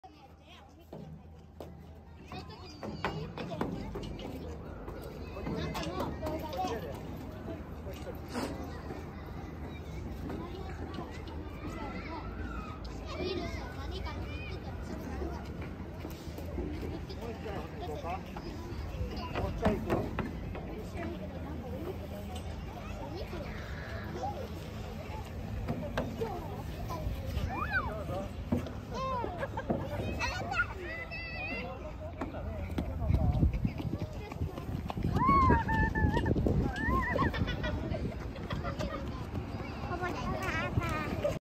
HOTARUGAIKE PARK, TOYONAKA, OSAKA
One of the best public park near Hotarugaike station in Toyonaka, Osaka, Japan